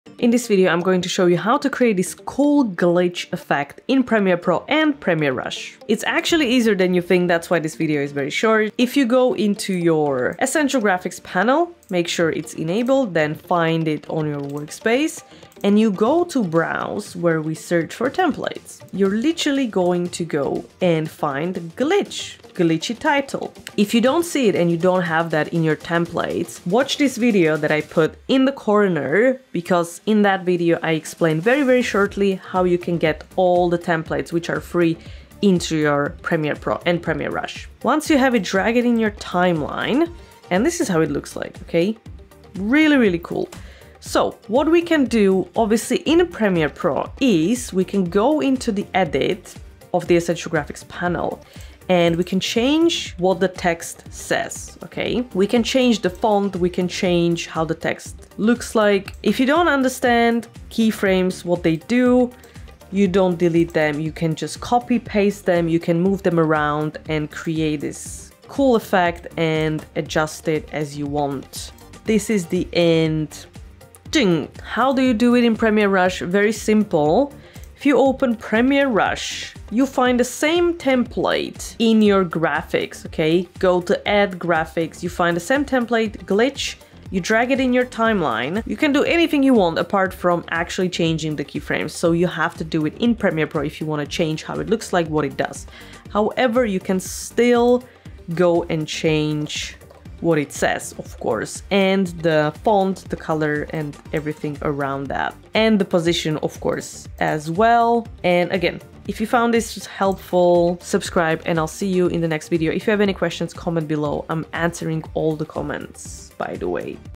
How to Create a Glitch Title in Premiere Rush and Pro?
Learn how to manipulate effects, tweak settings, and create a stunning glitch title that will captivate your audience.